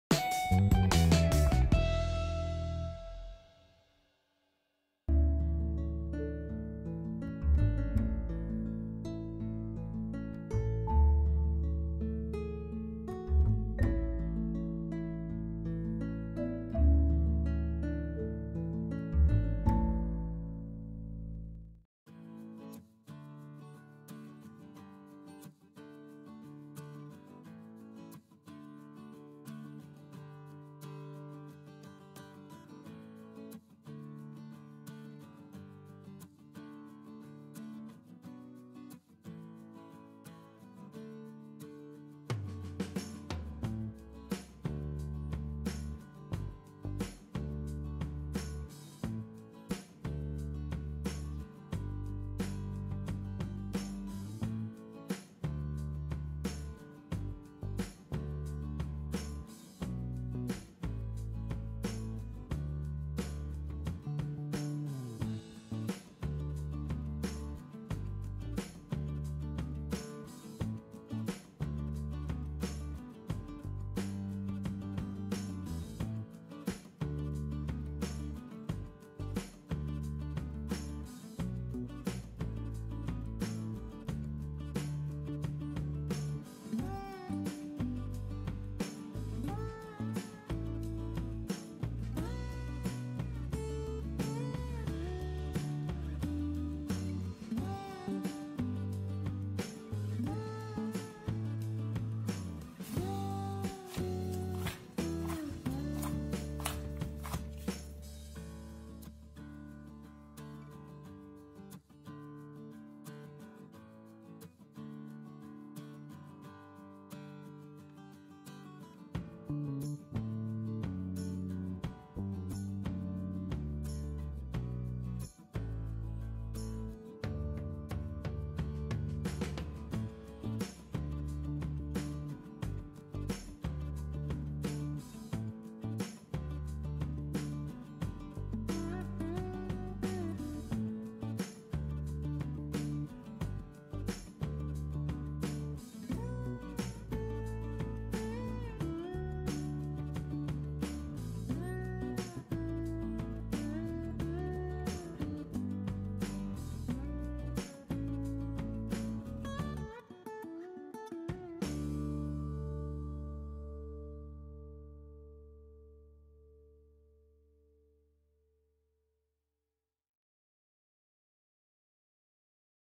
Easy Meatballs Recipe | Resepi Bebola Daging | 미트볼
Meatballs are easy and fun to make from scratch. This recipe is very simple and uses just a few ingredients.

Ingredients / Bahan-bahan:

500gm  Minced Meat / Daging Cincang
350gm  Flour / Tepung
150gm  Potato Starch / Tepung Ubi
1 tbsp   Baking Powder / Serbuk Penaik
40gm    Salt /Garam
10gm    White Pepper Powder / Lada Sulah
100ml   Water /Air

• Thanks a million for watching our videos!
• Don't forget to leave a comment so we know you were here before.
• We truly appreciate it!